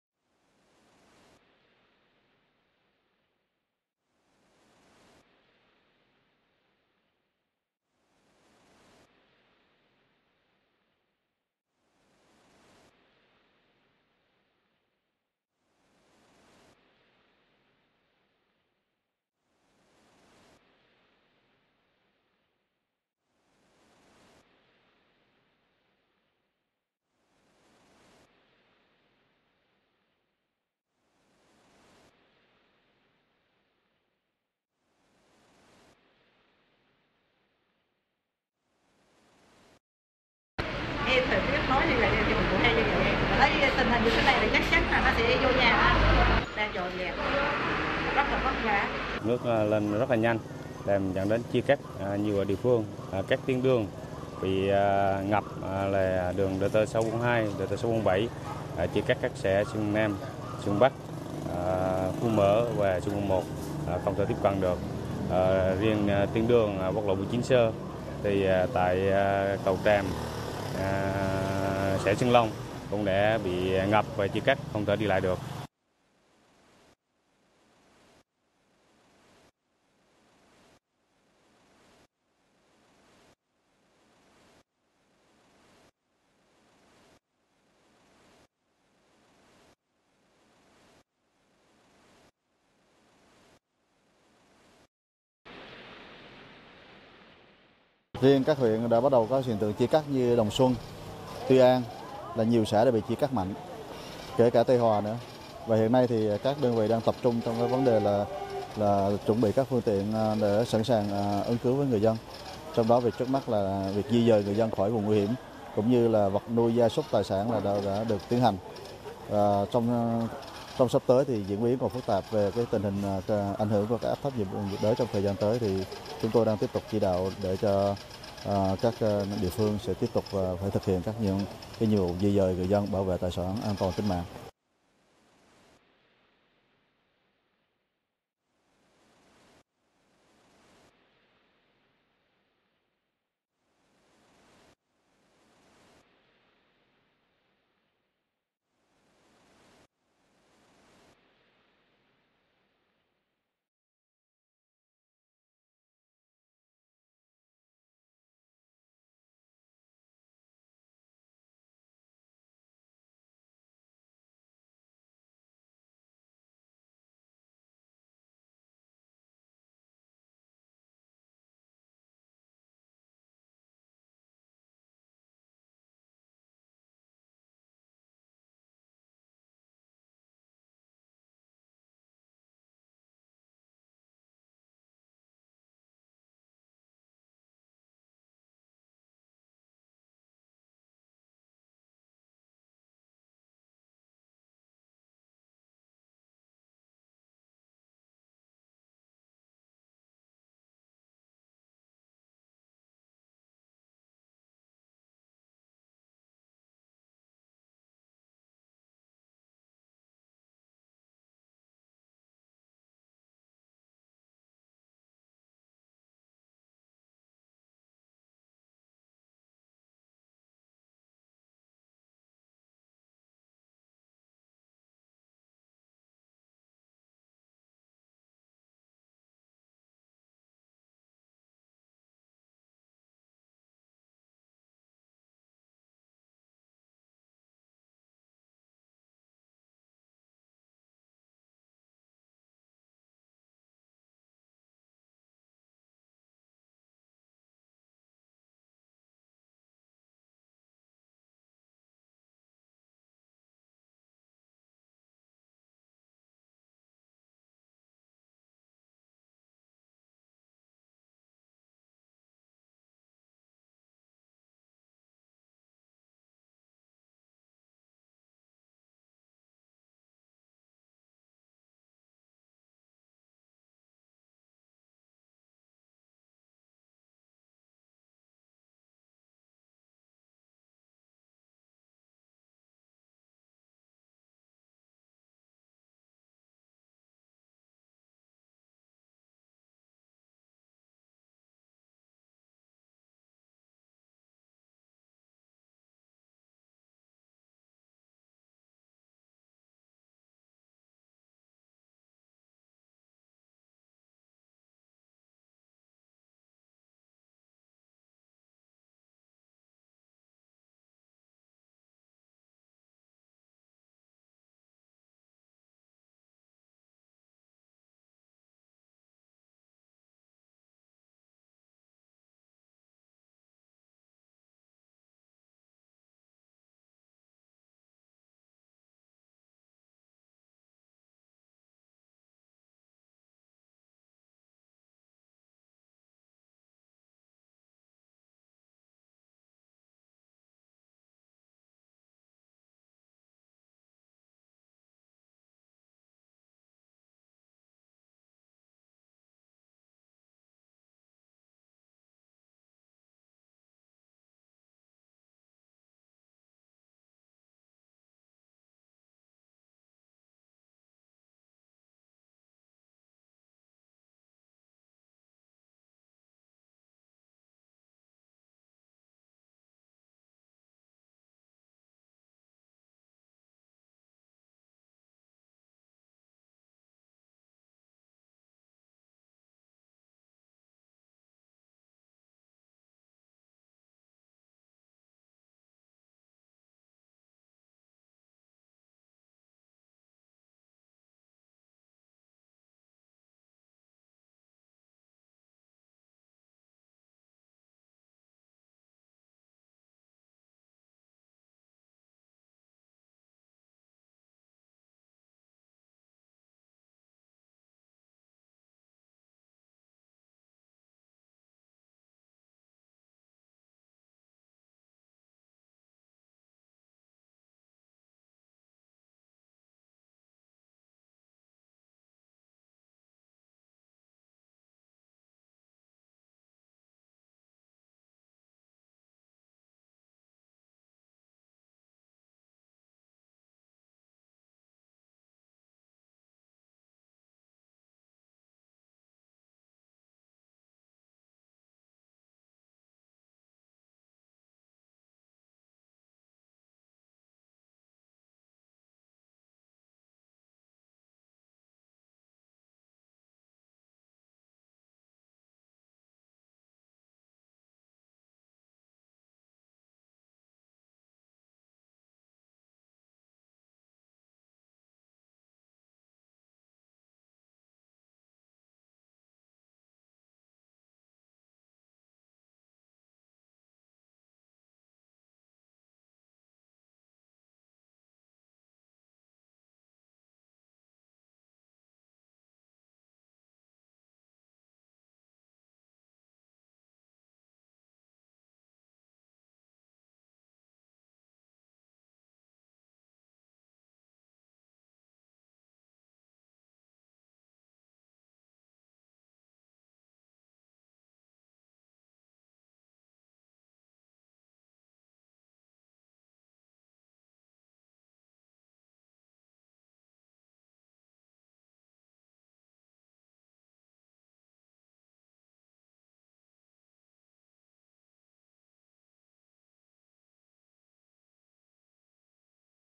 Flood situation in Phu Yen
The floods in the past few days made many areas in Phu Yen,

Play Video
Play
Current Time
0:00
Loaded: 0%
Progress: 0%
Duration Time
0:00
Mute
Quality
HD
Fullscreen

Heavy rains continued in the day 31-10 to 1-11 am causing many areas in Phu Yen province floods were divided, flooded. Noon 1-11, Ba River hydropower begins to discharge flood.

In Dong Xuan district, Ky Lo river water rises, DT 641 flooded many places, cut off traffic from the plain to Dong Xuan district.

In the town of La Hai (Dong Xuan district), the Ky Lo river rises, flooding the La Hai bridge over 1.5m; Cay Sung River is located on road 642, crossing Xuan Son North, water over 2m.

Flood water has cut off the road from Xuan Son Bac (Dong Xuan district) can not go down Xuan Tho 2 (Song Cau town).